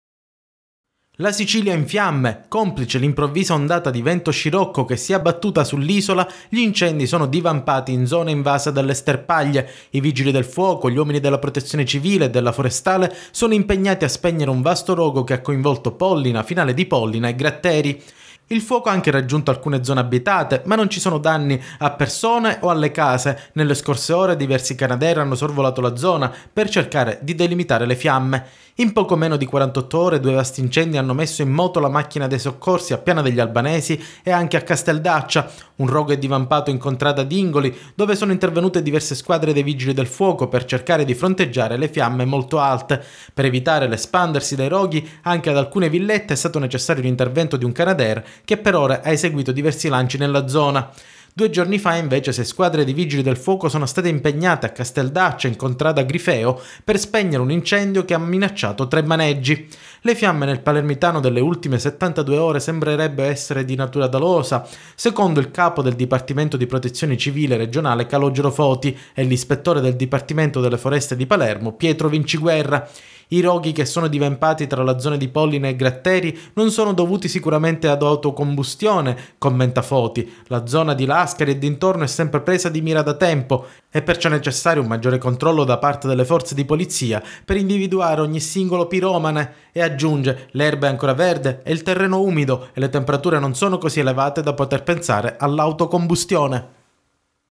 Scirocco e primi incendi: sulle Madonie fuoco vicino alle case, chiuso tratto dell'autostrada
Torna a soffiare il vento di scirocco in Sicilia e incendi divampano soprattutto in zone invase dalle sterpaglie. I vigili del fuoco, gli uomini della protezione civile e della forestale sono stati impegnati a spegnere un vasto rogo che ha coinvolto Pollina, Finale di Pollina e Gratteri.

Il fuoco, a partire dalle 18, ha lambito alcune zone abitate, a Pollina nella notte è stato evacuato un intero condominio in viale della Madonie. Problemi anche in campagna dove alcune fattorie sono state sgomberate. Danni anche ad un traliccio dell'Enel e ad un ripetitore. Ieri in serata i canadair hanno sorvolato la zona per cercare di delimitare il fronte del rogo. Le fiamme, andate avanti per tutta la notte, sono state spente completamente questa mattina.

Intanto è stata chiusa la A20 nel tratto tra Castelbuono e Tusa direzione Palermo, traffico lungo la Statale 113 dove sono stati deviati i mezzi. I canadair per spegnere l'incendio hanno gettato una schiuma che ha reso scivoloso il manto stradale, al lavoro gli operai dell'Anas.

Ma già da 48 ore due vasti incendi hanno messo in moto la macchina dei soccorsi a Piana degli Albanesi e a Casteldaccia. Un rogo è divampato in contrada Dingoli, dove sono intervenute diverse squadre dei vigili del fuoco per cercare di fronteggiare le fiamme molto alte. Per evitare l'espandersi dei roghi anche ad alcune villette è stato necessario l'intervento di un canadair che per ore ha eseguito diversi lanci nella zona.
Mentre, due giorna fa sei squadre di vigili del fuoco sono state impegnare a Casteldaccia in contrada Grifeo per spegnere un incendio che ha minacciato tre maneggi. (gds)